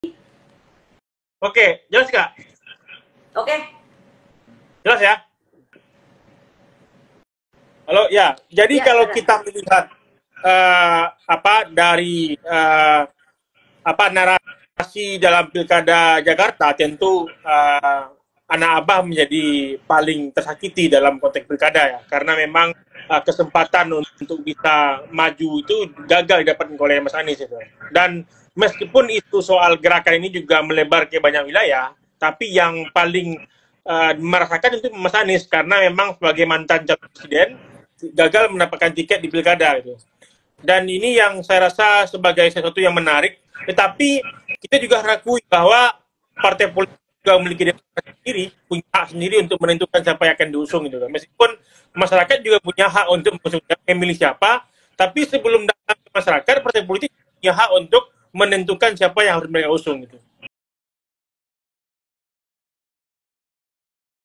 GERAKAN COBLOS 3 PASLON, PENGAMAT: BENTUK KEKECEWAAN ANAK ABAH
Direktur Eksekutif Aljabar Strategic, Arifki Chaniago, menyatakan bahwa munculnya gerakan untuk mencoblos tiga pasangan calon (paslon) merupakan bentuk kekecewaan dari anak-anak Abah. Ia menilai bahwa sebagai mantan calon presiden yang gagal mendapatkan tiket untuk maju dalam pemilihan kepala daerah DKI Jakarta, hal ini sangat mengecewakan.

Arifki Chaniago juga menegaskan bahwa partai politik memiliki hak untuk menentukan calon yang akan diusung.